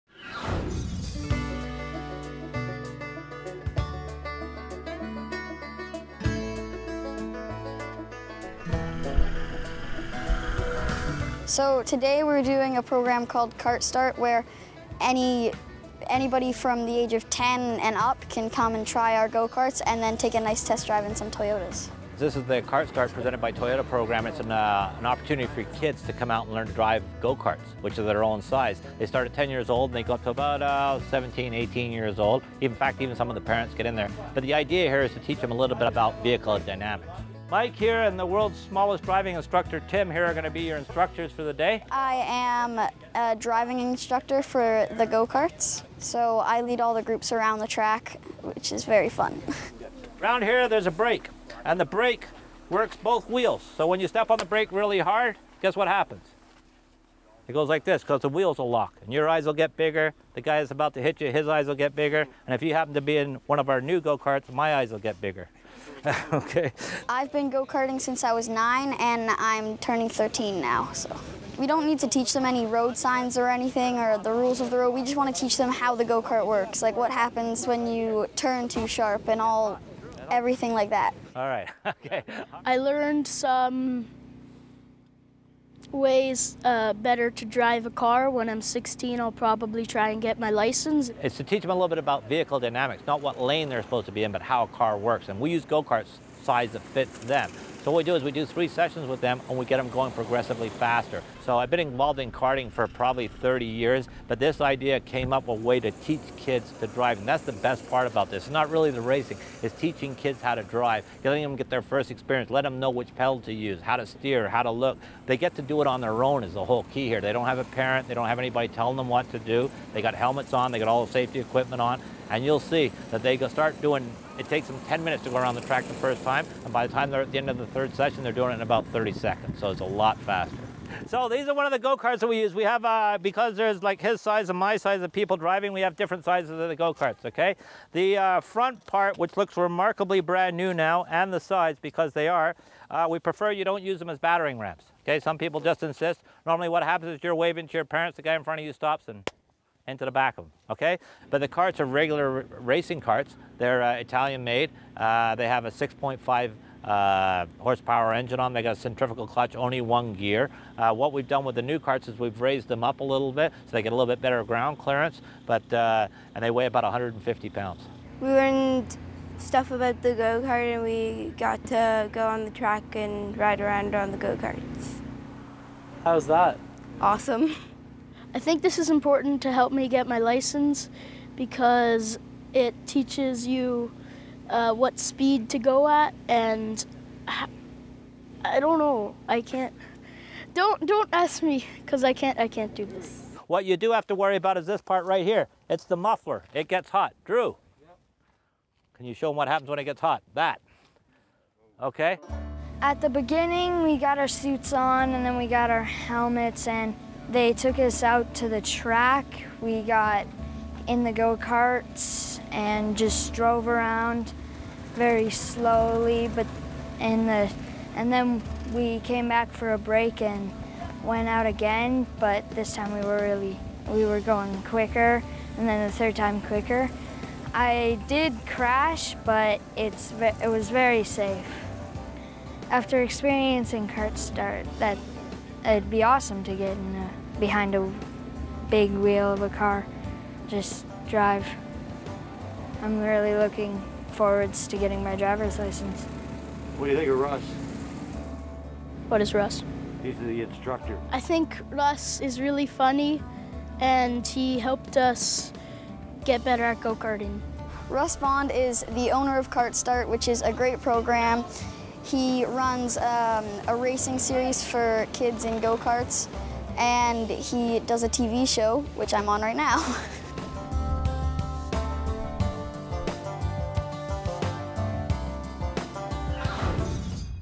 Motoring TV visits Kart Start driving education program
The Motoring TV team spent the day visiting Kart Start, a driving education program run by Two-Minute Test Drive host Russ Bond.

Open to children ages 10 and up, the program teaches the importance of road safety and vehicle dynamics using go karts and a team of experienced instructors.